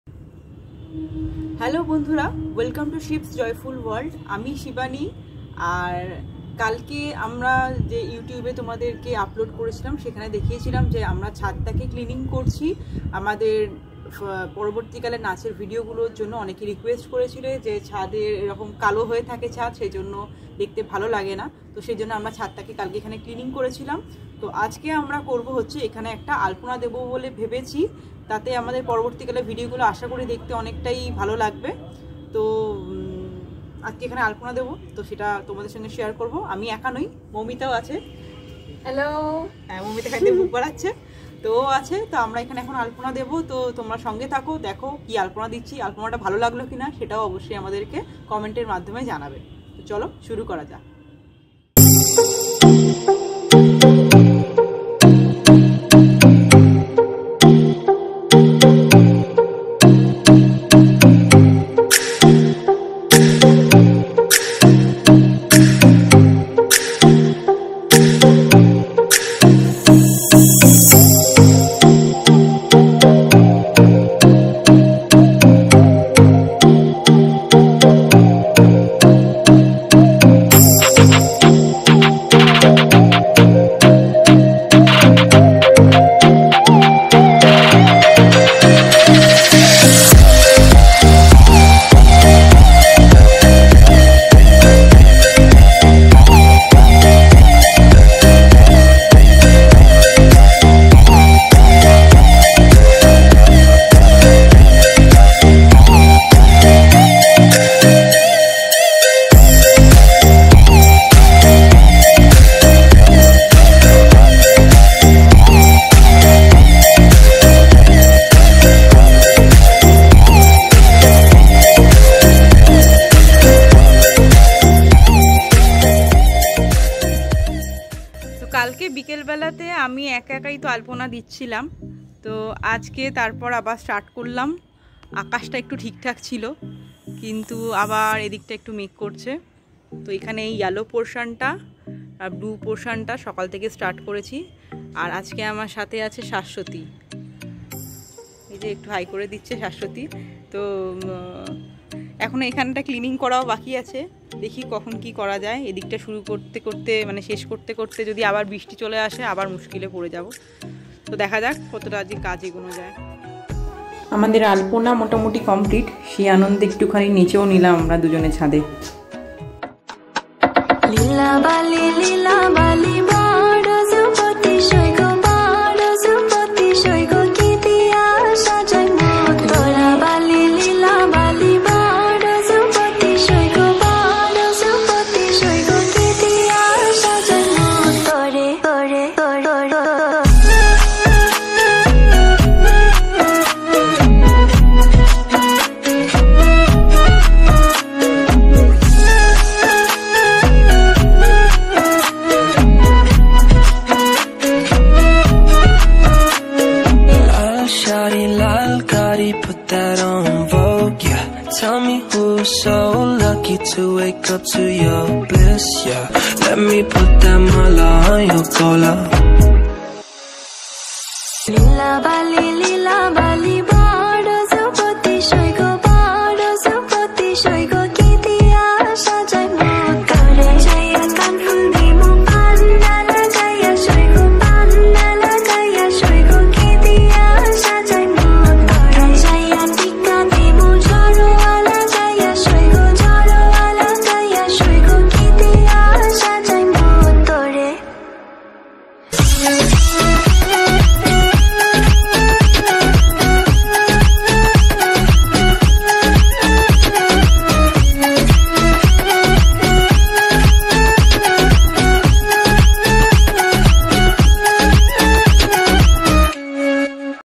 আমরা ছাদে আলপনা দিলাম || মৌমিতা এটা কি করলো😂 || alpona special vlog
alpona design @shibsjoyfulworld